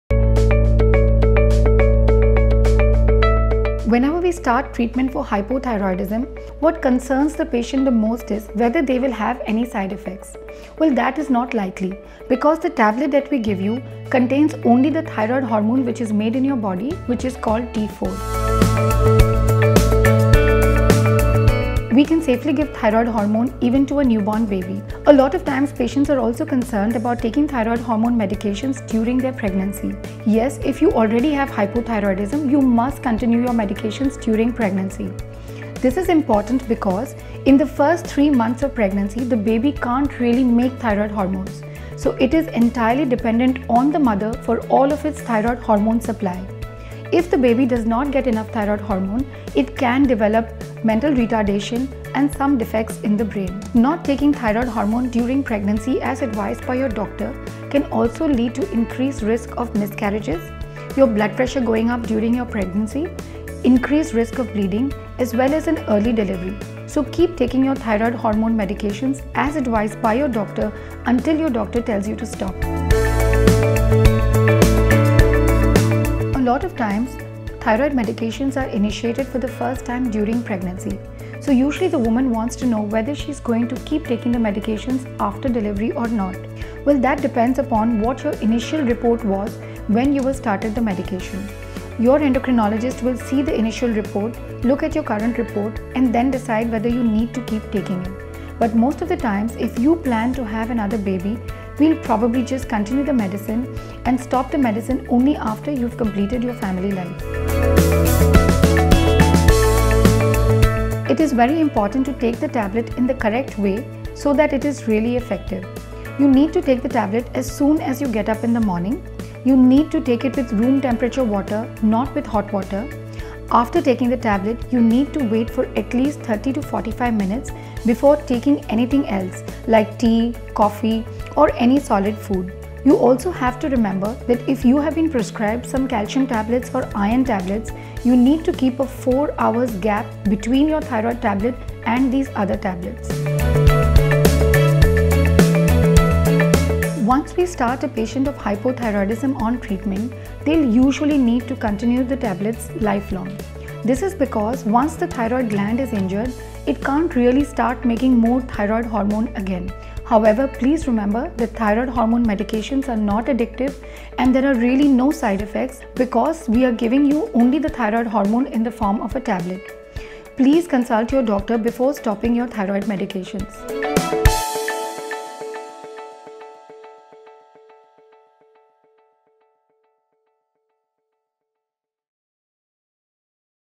Medicines for Hypothyroidism are safe for life
Patients are often scared about the prospect of taking thyroid medication for life. Dr Tejal Lathia, an Endocrinologist practising in Navi Mumbai sheds light on this and more.